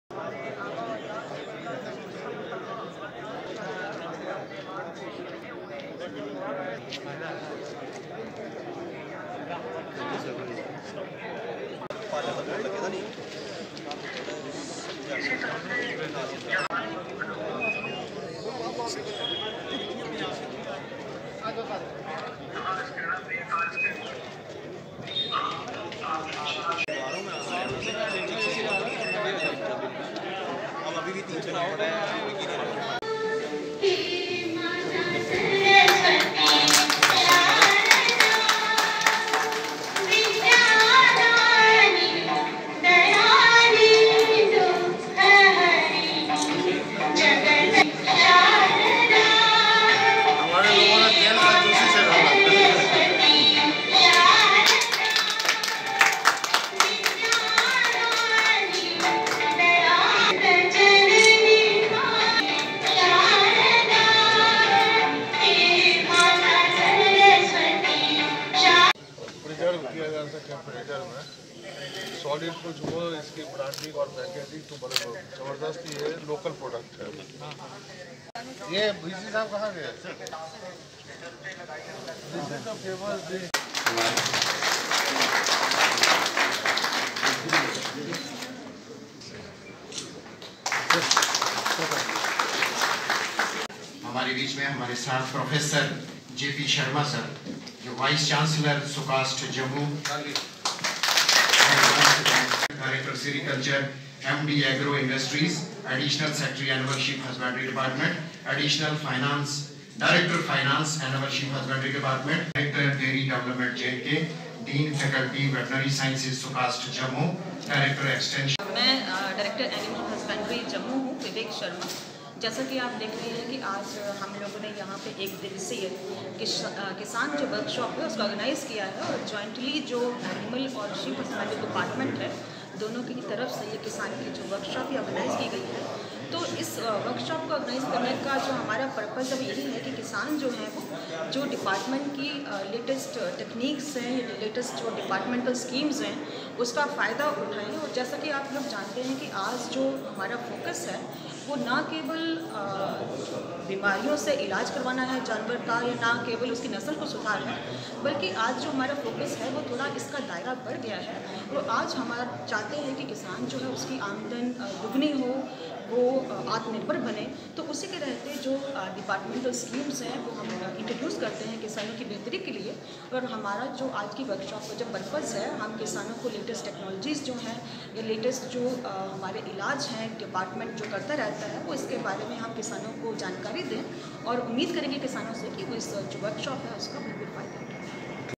Department of Animal and Sheep Husbandry hold One Day Farmer's Workshop at Kissan Ghar Talab Tillo
Department of Animal and Sheep  Husbandry hold One Day Farmer's Workshop at Kissan Ghar Talab Tillo Jammu.
Report
By

RK